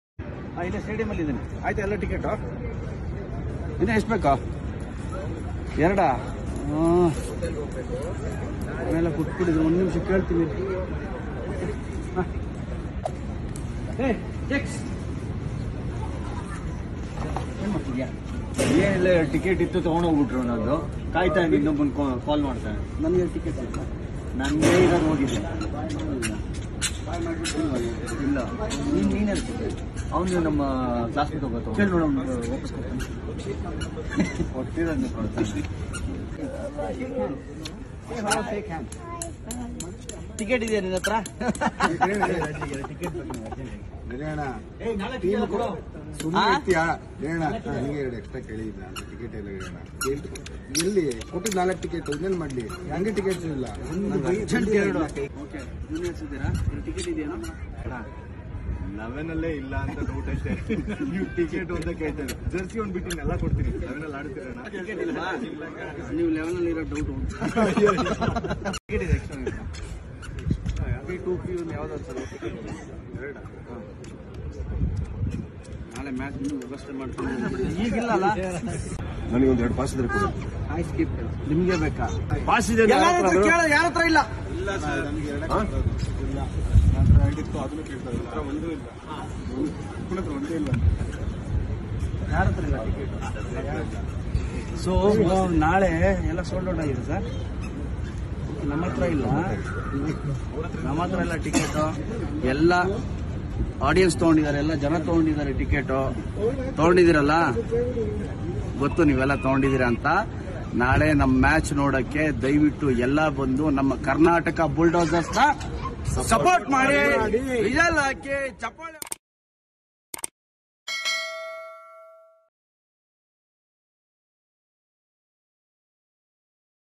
2 ಟಿಕೆಟ್ ಗಾಗಿ ಪರದಾಡುತ್ತಿರುವ ಸುದೀಪ್ ಮತ್ತು ಗಣೇಶ್ | Sudeep and Ganesh CCL tickets comedy | Kannada Tv
karnataka Bulldozers sudeep and ganesh comedy. karnataka bulldozers vs chennai rhinos 2023 tickets comedy.

ganesh comedy in kcc 04th Mar2023   KannadaTv